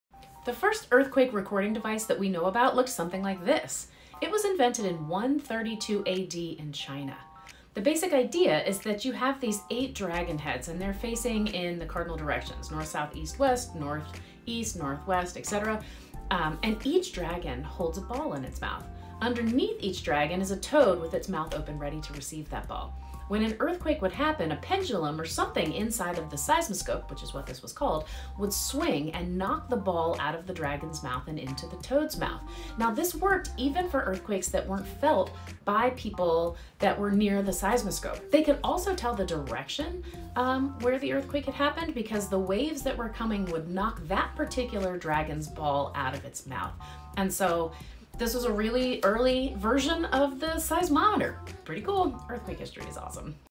Seismoscope - the 1st seismometer!
The 1st earthquake detection machine was called a seismoscope and it was invented in China in 132 AD!